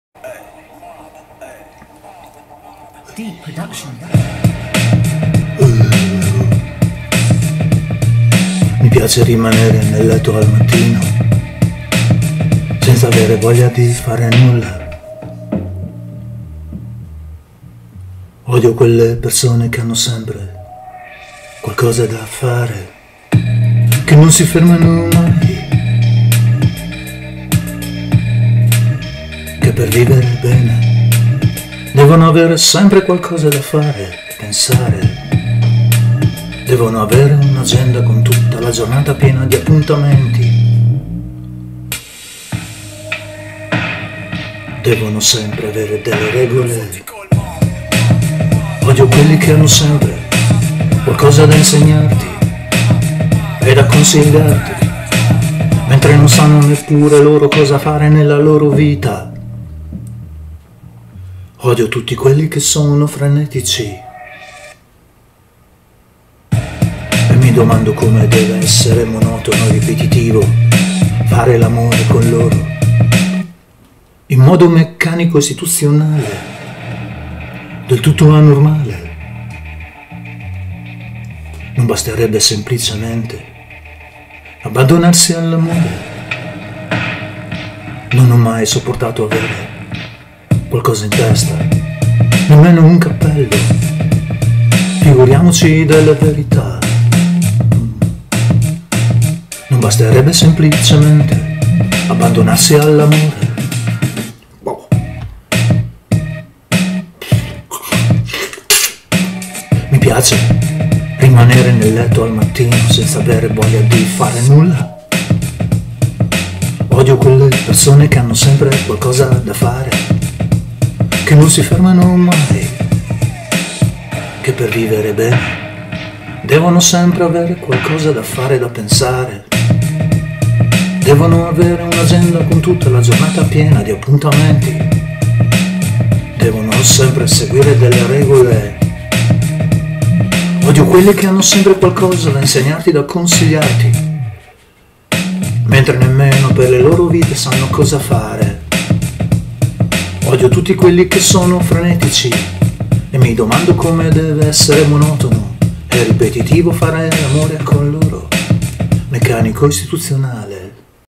Mi piace rimanere nel letto al mattino - Roberto Piana
Mi piace rimanere nel letto al mattino senza avere voglia di fare nulla.
Odio quelle persone che hanno sempre qualcosa da fare,  che non si fermano mai, che per vivere bene devono avere sempre qualcosa da fare e pensare,  devono avere un'agenda con tutta la giornata piena di appuntamenti, devono sempre avere delle regole.  Odio quelli che hanno sempre qualcosa da insegnarti e da consigliarti mentre non sanno nemmeno loro cosa fare per le loro vite. Odio tutti quelli che sono frenetici e mi domando come deve essere monotono e ripetivo fare l'amore con loro in modo meccanico,  istituzionale. Non basterebbe semplicemente abbandonarsi all'amore? Non ho mai sopportato avere qualcosa in testa, nemmeno un cappello, figuriamoci delle verità!